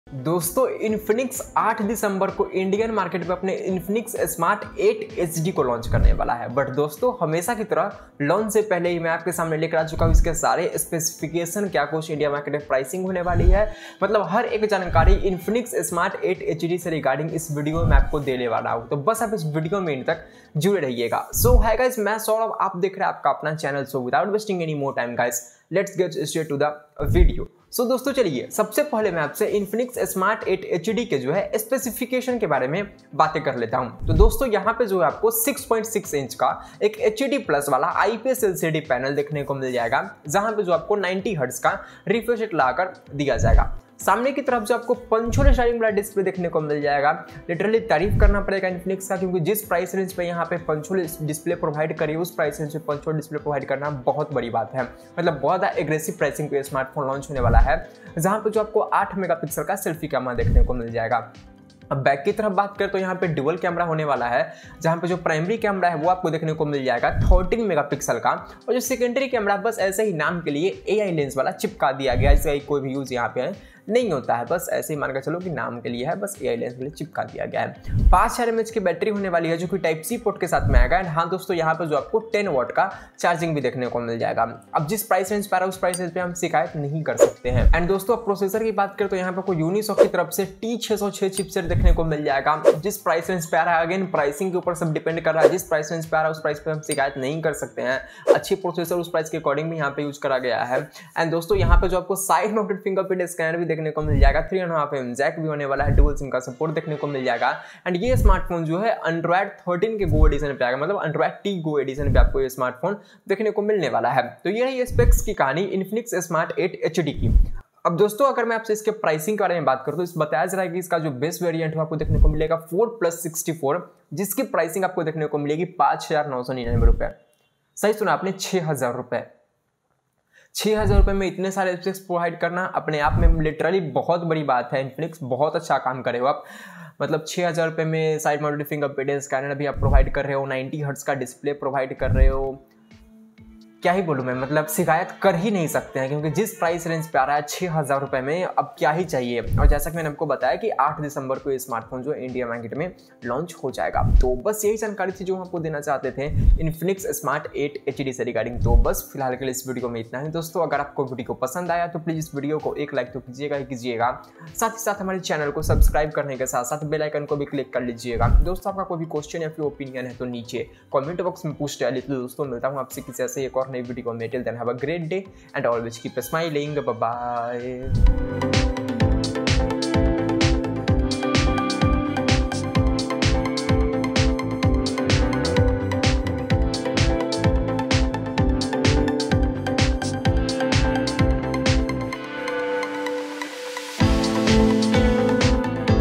Infinix Smart 8 HD Launch Date In India | Infinix Smart 8 HD India Price, Specifications, Camera,SOC
Hello Dosto, Infinix 8 December ko India mein Infinix Smart 8 HD ko launch karne wala hai jisme aapko Unsicoc T606 SOC, 8 M.P Selfie Camera, 13 M.P Dual Rear Camera, 5000 Mah Battery, 10 Watt Charging, 90 Hz HD Display aur bhi bhot kuch dekhne ko milta hai aur sath hi maine aapse Infinix Smart 8 HD ke India Pricing ke baare mein bhi baat kiya hai to poori jankari ke liye iss video ko end tak dekhe..